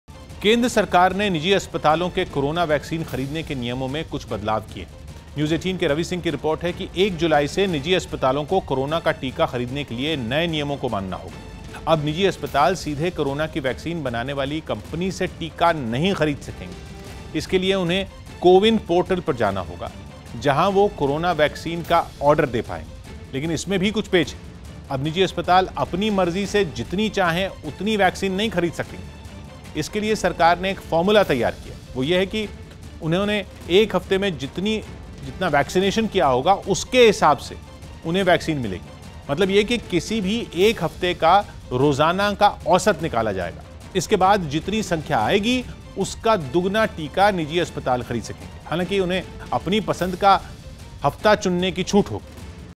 केन्द्र ने निजी अस्पतालों के Corona Vaccine ख़रीदने के नियमों में किये बदलाव, 1 जुलाई से नियम लागू
केन्द्र सरकार ने निजी अस्पतालों के कोरोना वैक्सीन ख़रीदने के नियमों में कुछ बदलाव किए. 1 जुलाई से निजी अस्पतालों को कोरोना वैक्सीन खरीदने के लिए नए नियम मानने होंगे.